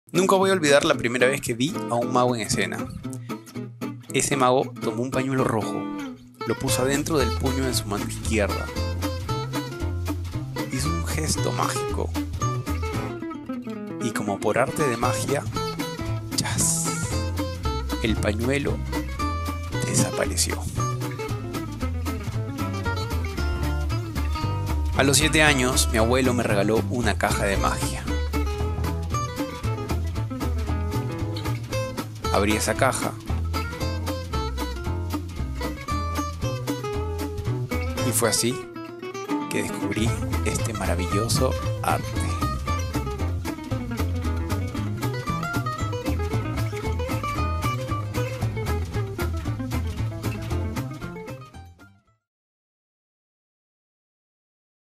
LA PRIMERA VEZ.
Mi primer contacto con la magia fué a la edad de 7 años, con la típica caja de magia que me regalo mi abuelo Eduardo.
Nunca olvidaré cuando vi por a un mago por primera vez en escena.
Fué en la kermesse de mi colegio, ese mago tomó un pañuelo y lo desapareció entre sus manos.
Con ese misterio en mi cabeza,  abrí esa caja, y me sumergí en el mundo del
ARTE de la MAGIA.